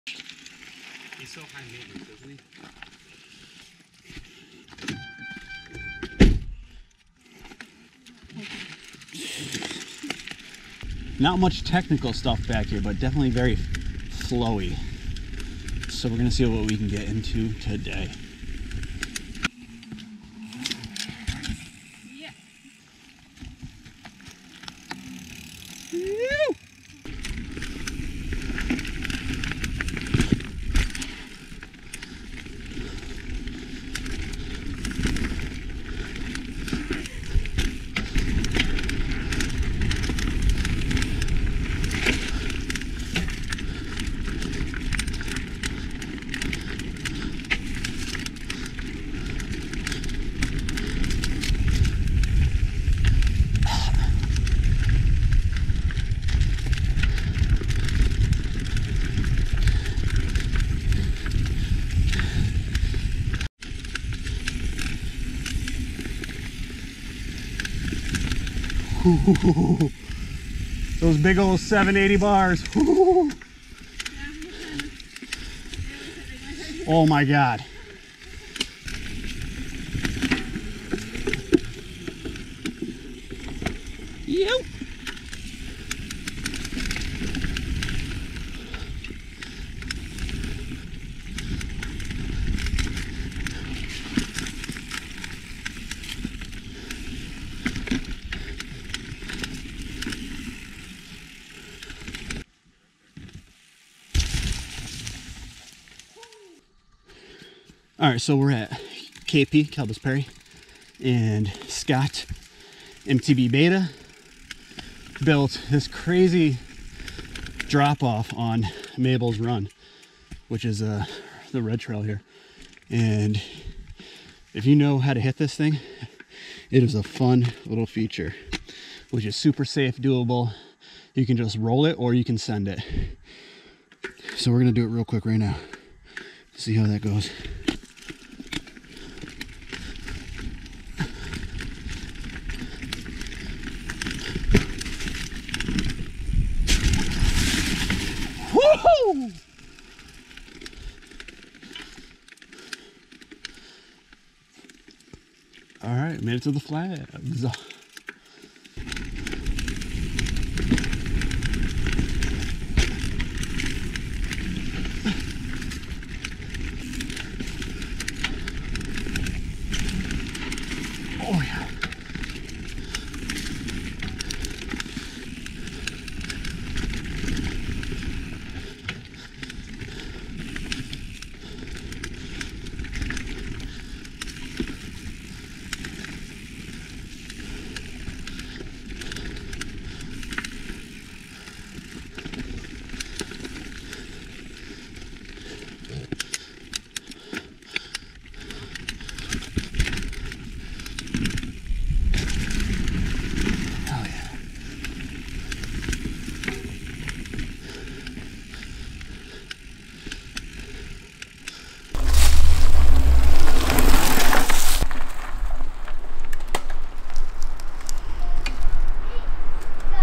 Kalabus Perry Teaser MTB 2020 | 4K
A quick look at KP trails located in Saratoga NY will show you how much flow this place has. Very fast and easy to navigate. New safe and sendy features are being added frequently! Keep an eye out for a full coverage video of Kalabus Perry trails to be dropped soon!

The Beta Drop - Building was blessed by landowners permission under certain guidelines - please do not build without.

Merch we use:
GoPro Hero 7 Black bundle
GoPro Karma Grip Gimbal
GoPro Chesty
CYGOLITE Dash 460 for helmet light
CYGOLITE Metro 700 Bar light